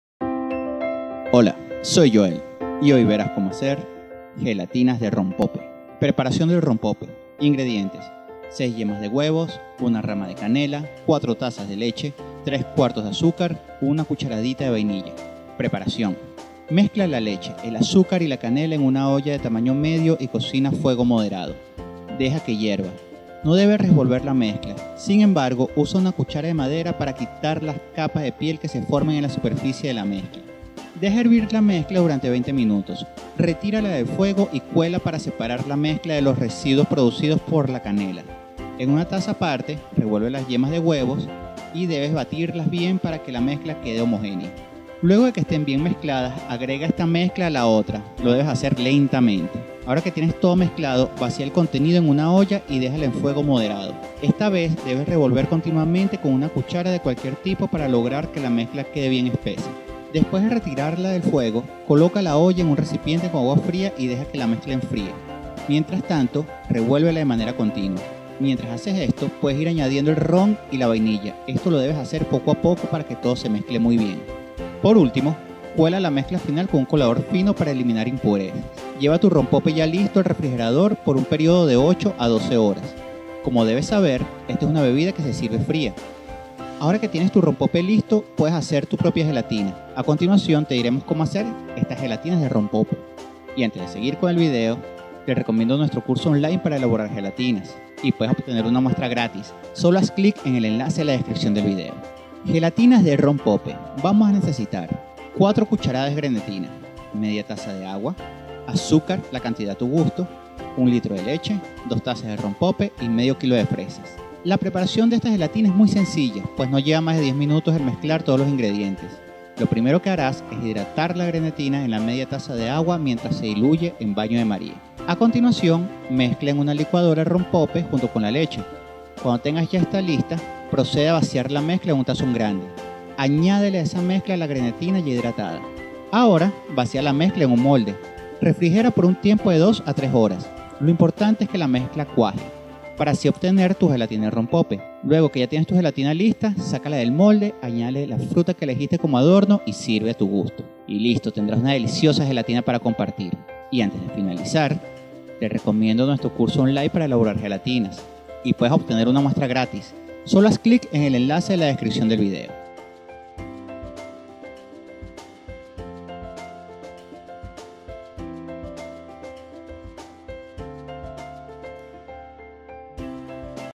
RECETAS DE GELATINA DE ROMPOPE PASO A PASO
Sabemos que el rompope es una bebida muy deliciosa pero si la haces con gelatina es mucho mas,  hoy te enseñamos rápidamente las recetas de gelatina de rompope paso a paso, no solo aprenderás a preparar el rompope sino la gelatina también, espero te guste!